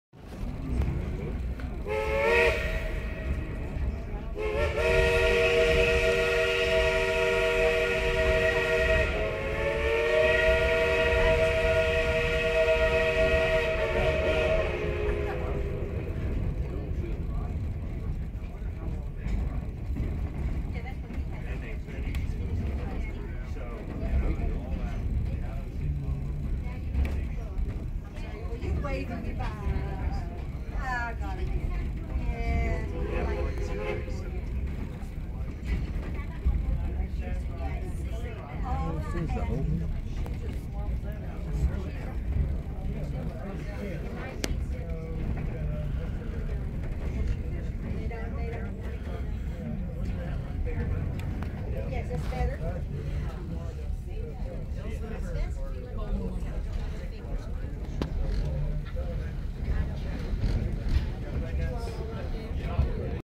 Passing The Old Cass Sawmill
Back in it's heyday, Cass was bustling Logging Town, here is what remains of the Old Cass Sawmill